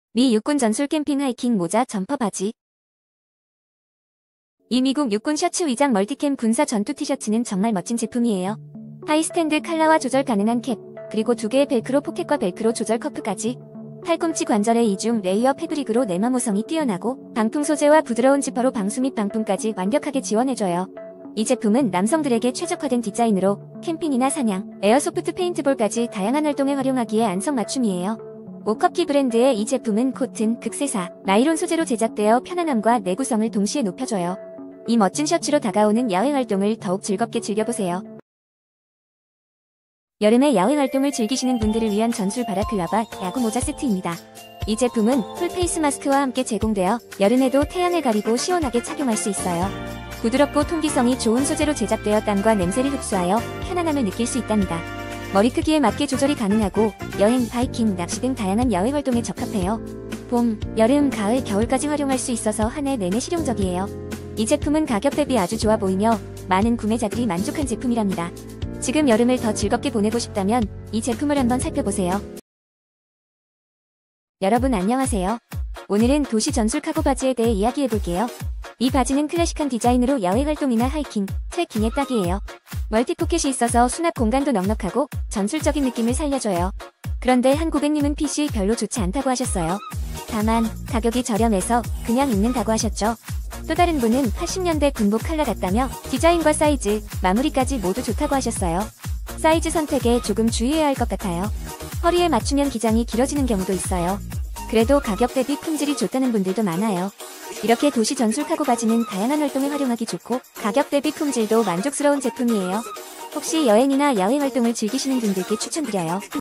미 육군 전술 캠핑 하이킹 트래킹 바라클라바 모자 점퍼 바지
00:00 미 육군 전술 캠핑 하이킹 모자 점퍼 바지

00:04 BEST 1. 미국 육군 셔츠 위장 멀티캠 군사 캠핑 점퍼
🔥특가 : 13,596원 (68%)
202 리뷰 | 1,000+ 누적 판매

00:38 BEST 2. 전술 바라클라바 야구 캠핑 모자
🔥특가 : 3,529원
149 리뷰 | 800+ 누적 판매

01:10 BEST 3. 전술 카고 바지 클래식 야외 하이킹 트레킹 캠핑 바지
🔥특가 : 6,718원 (79%)
5,188 리뷰 | 10,000+ 누적 판매

👉 본 영상은 채널의 주관적인 생각이 포함되어 있습니다.
👉 알리익스프레스 제휴로 활동에 따라 일정액의 수수료를 제공받을 수 있습니다.
👉 제품에 대하여 채널은 책임을 지지 않으며, 제품 관련은 알리익스프레스 고객센터로 연락 바랍니다.